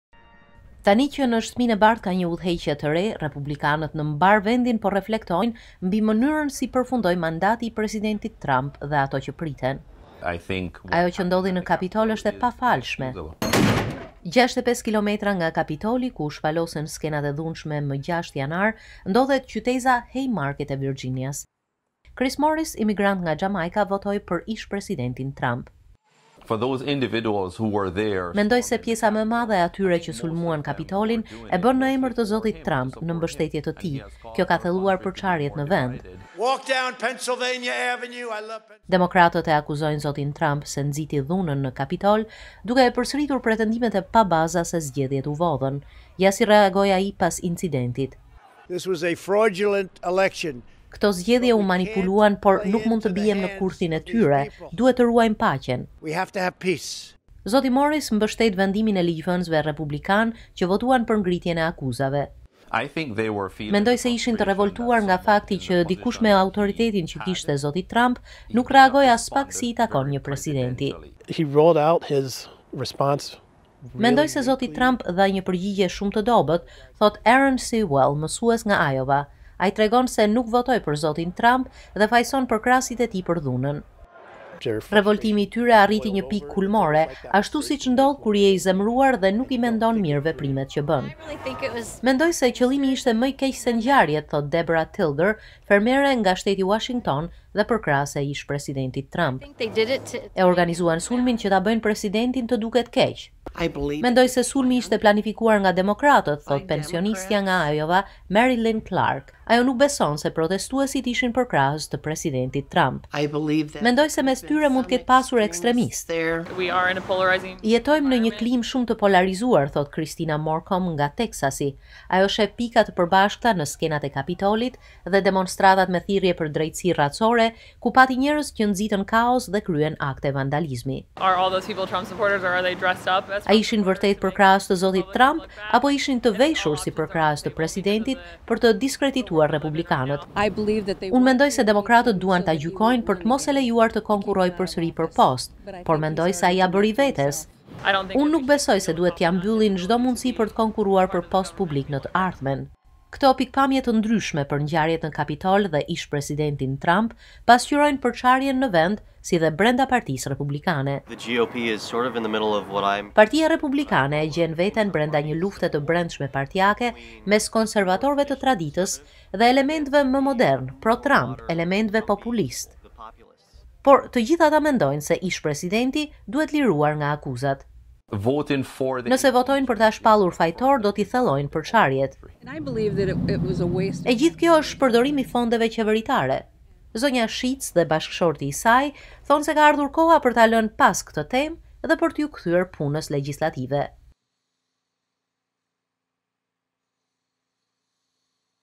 Pikëpamjet e ndryshme mes republikanëve pasqyrojnë përçarjet brenda partisë
Përfundimi dramatik i mandatit të Presidentit Trump dhe gjyqi i pritshëm kundër tij e kanë përçarë Partinë Republikane. Por jo të gjithë ata që votuan për zotin Trump prezantohen si republikanë dhe jo të gjithë republikanët thonë se mbështesin zotin Trump.